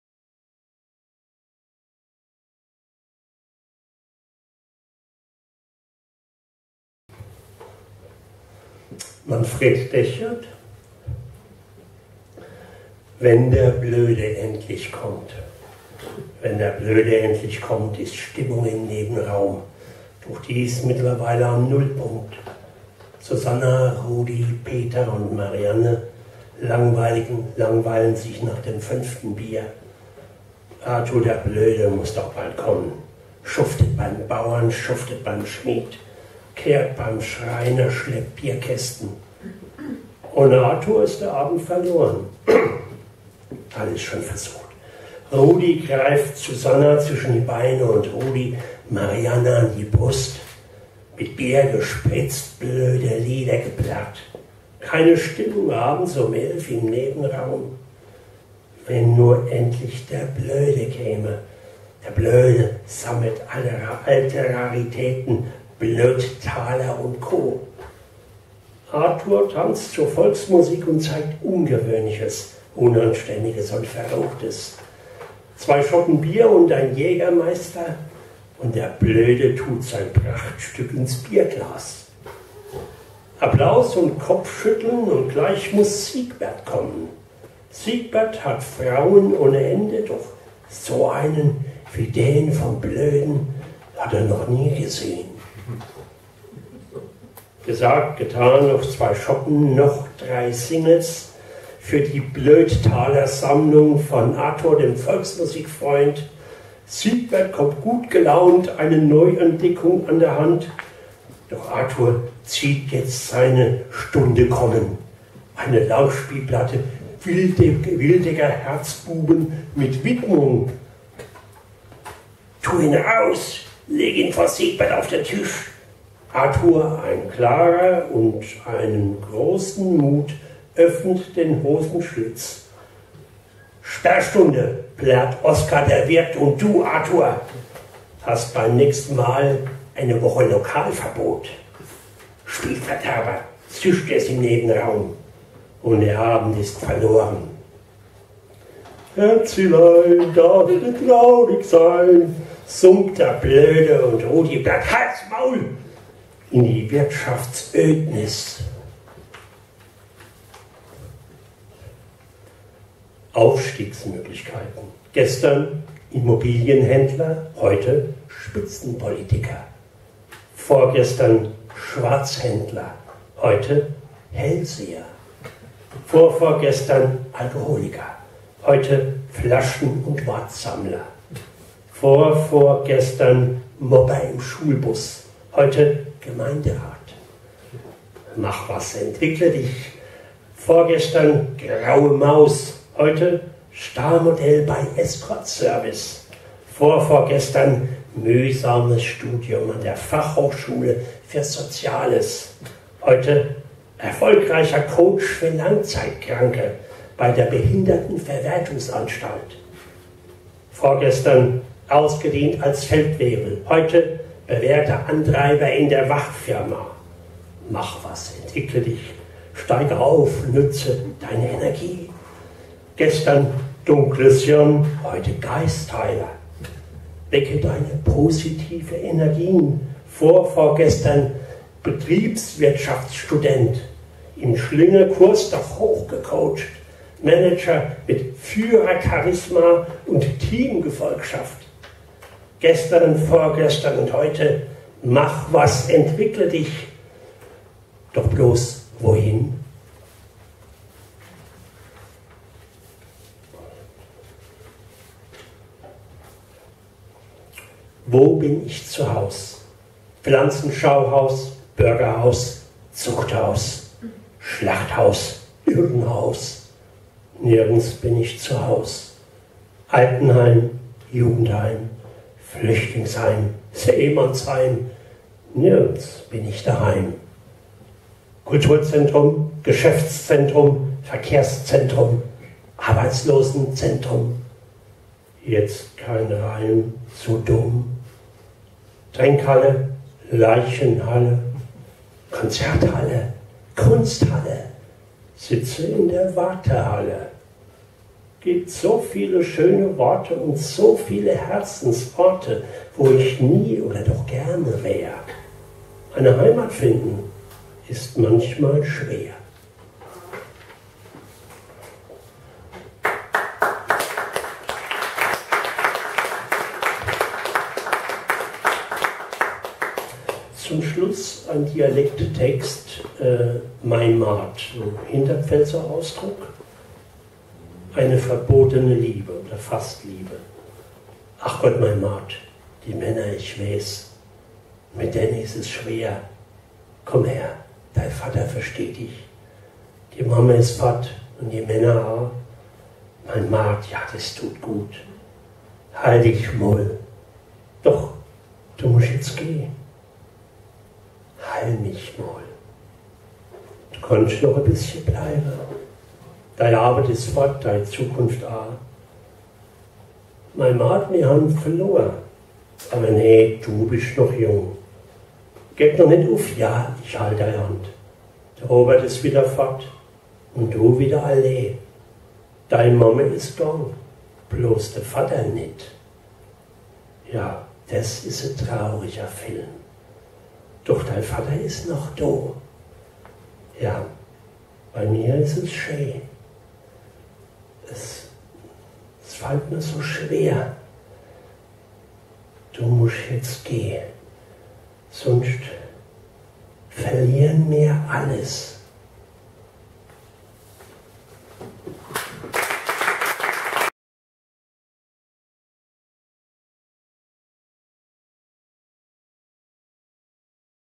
Ist der Ruf erst ruiniert, lebts sich leicht und ungeniert - Manfred Dechert
Lesung der Autorengruppe "Dichterzusammen" im Sol y Sombra, Ludwigshafen zu dem Thema "Ist der Ruf erst ruiniert, lebts sich leicht und ungeniert"
Es liest Manfred Dechert